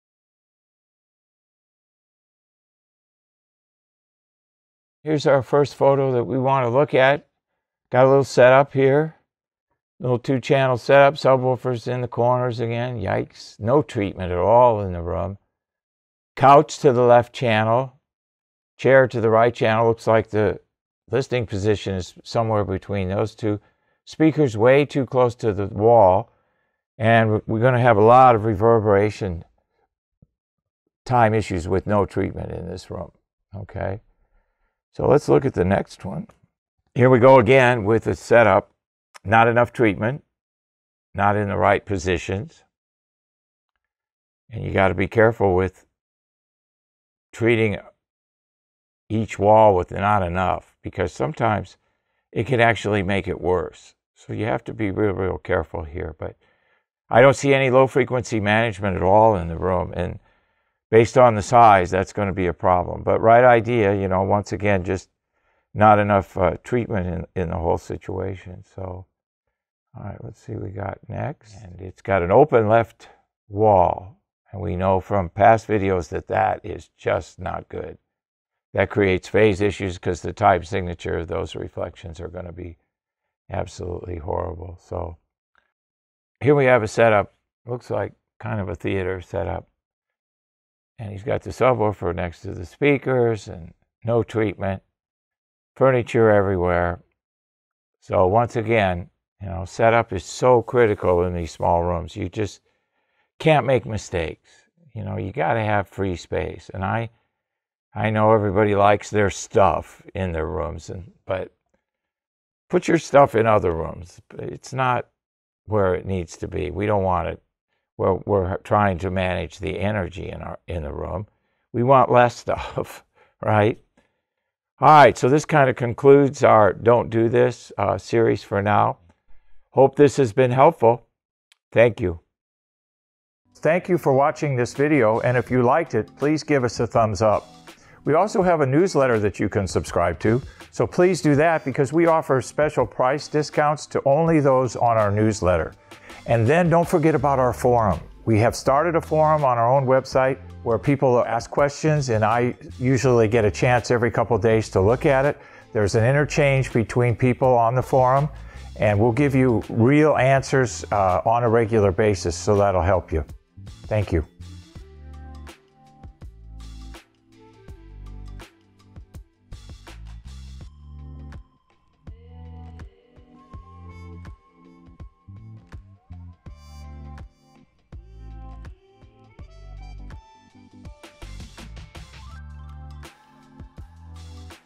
Don't Do This! # 17
- - This is part 17 of our "Don't do this" series which discusses common mistakes in room acoustics. Watch the video to find out more!